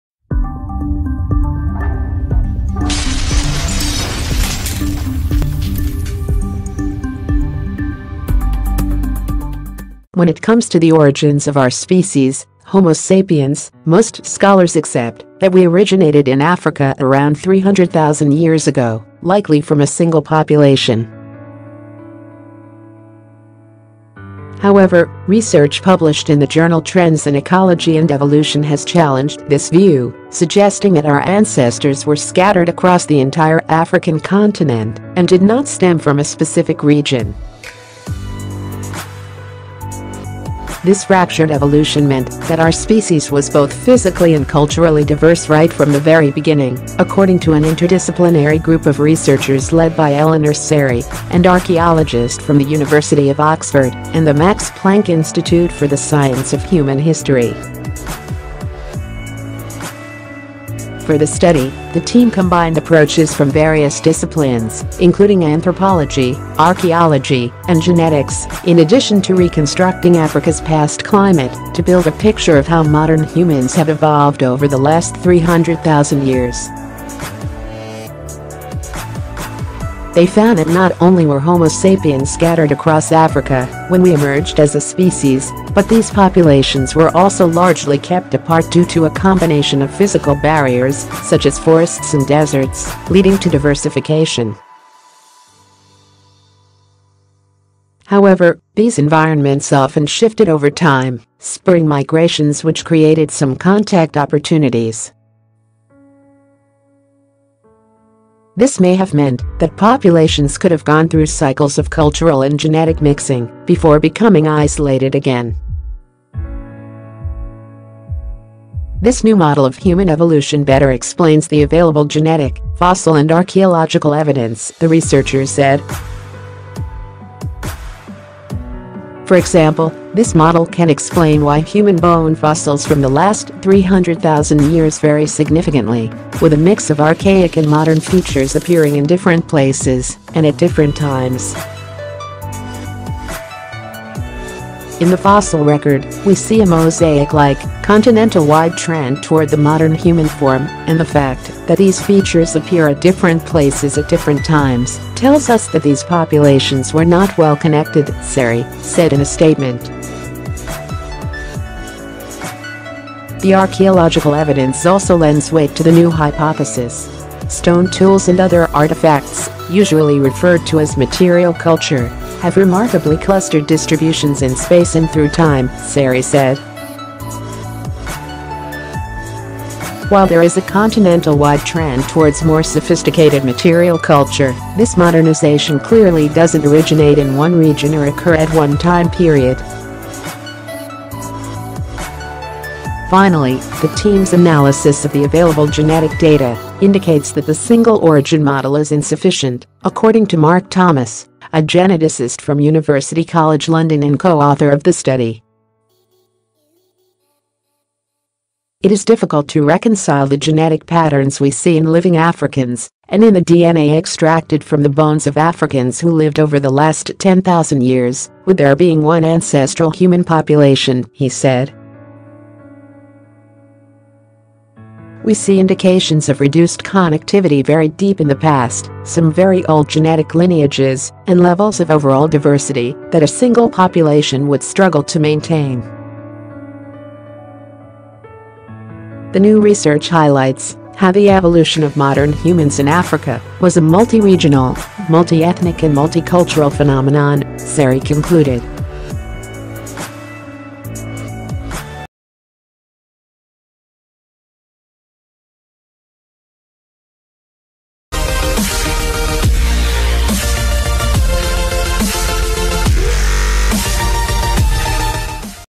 Humans Did Not Evolve from a Single Population in Africa, Researchers Say | Gift Of Life
When it comes to the origins of our species, homo sapiens, most scholars accept that we originated in Africa around 300,000 years ago, likely from a single population. However, research published in the journal Trends in Ecology & Evolution has challenged this view, suggesting that our ancestors were scattered across the entire African continent and did not stem from a specific region. This fractured evolution meant that our species was both physically and culturally diverse right from the very beginning, according to an interdisciplinary group of researchers led by Eleanor Scerri, an archaeologist from the University of Oxford and the Max Planck Institute for the Science of Human History. For the study, the team combined approaches from various disciplines, including anthropology, archaeology and genetics, in addition to reconstructing Africa’s past climate, to build a picture of how modern humans have evolved over the last 300,000 years. They found that not only were homo sapiens scattered across Africa when we emerged as a species, but these populations were also largely kept apart due to a combination of physical barriers, such as forests and deserts, leading to diversification. However, these environments often shifted over time, spurring migrations which created some contact opportunities. This may have meant that populations could have gone through cycles of cultural and genetic mixing before becoming isolated again. This new model of human evolution better explains the available genetic, fossil and archaeological evidence, the researchers said. For example, this model can explain why human bone fossils from the last 300,000 years vary significantly, with a mix of archaic and modern features appearing in different places and at different times. "In the fossil record, we see a mosaic-like, continental-wide trend toward the modern human form, and the fact that these features appear at different places at different times tells us that these populations were not well connected," Scerri, said in a statement. The archaeological evidence also lends weight to the new hypothesis. “Stone tools and other artifacts—usually referred to as material culture—have remarkably clustered distributions in space and through time,” Scerri said. “While there is a continental-wide trend towards more sophisticated material culture, this ‘modernization’ clearly doesn’t originate in one region or occur at one time period.”     Finally, the team’s analysis of the available genetic data indicates that the single origin model is insufficient, according to Mark Thomas, a geneticist from University College London and co-author of the study. “It is difficult to reconcile the genetic patterns we see in living Africans, and in the DNA extracted from the bones of Africans who lived over the last 10,000 years, with there being one ancestral human population,” he said. “We see indications of reduced connectivity very deep in the past, some very old genetic lineages, and levels